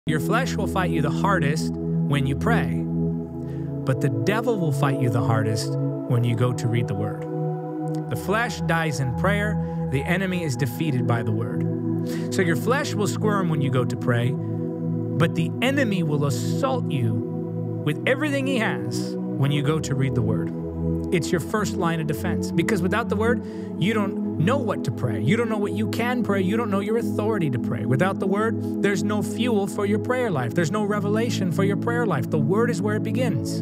How to Fight Against the Enemies of Self and Satan
As believers, we are in a daily battle against ourselves and the enemy. Our own strength alone isn’t enough to defeat these enemies. We must rely on the powerful Word of God.

You can watch the full sermon “How to Receive and Keep the Fire of the Holy Spirit” by clicking